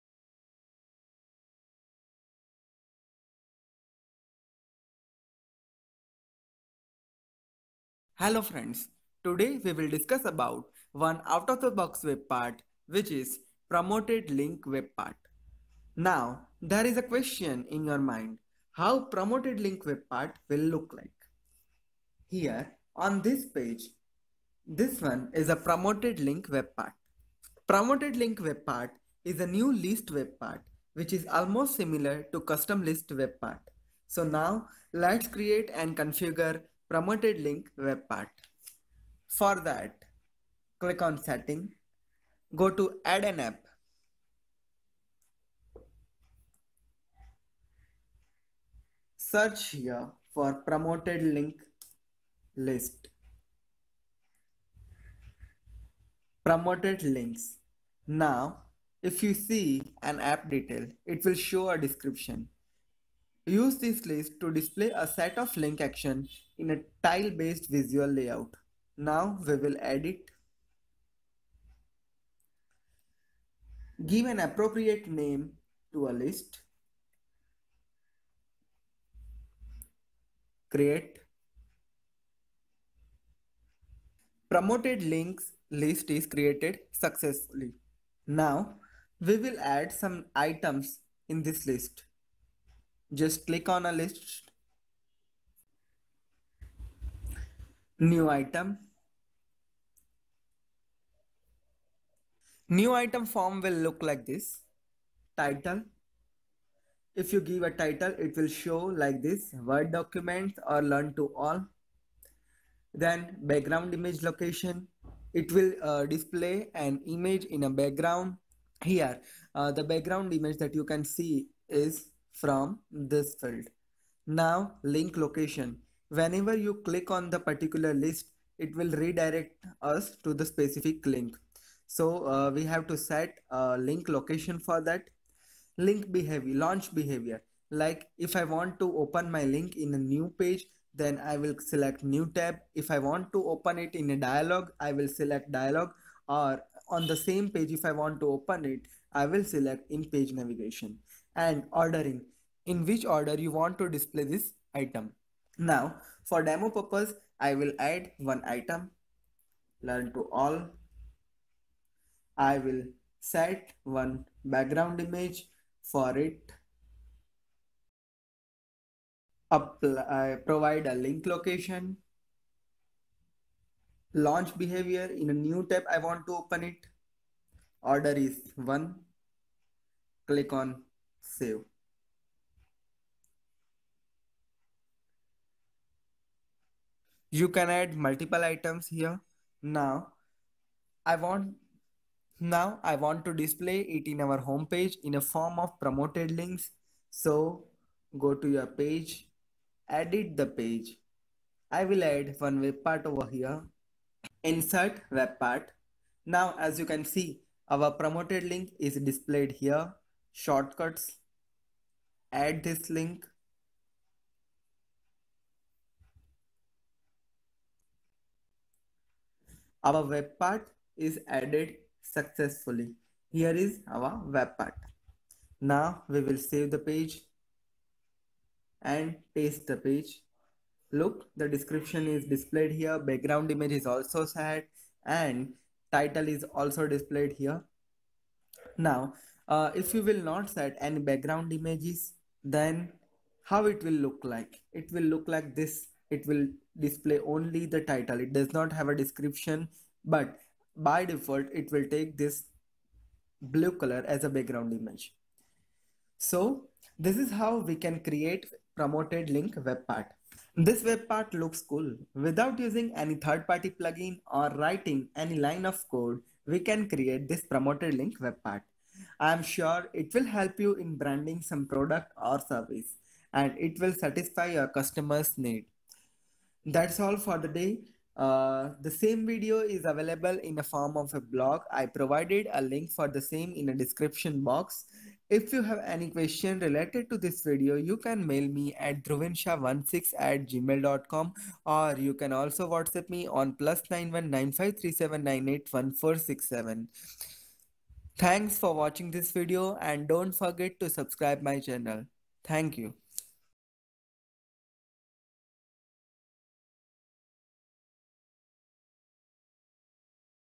Promoted Links WebPart In SharePoint
In this video we will learn how we can configure Promoted Links web part in SharePoint.
Promoted links webpart is a new list webpart and a way of adding items. This webpart is almost the same as custom list webpart
only the look and feel are different.This webpart provides a tile view of items, like documents, lists and libraries, items, and pages.

Vinodh N.

If you have any doubt related this video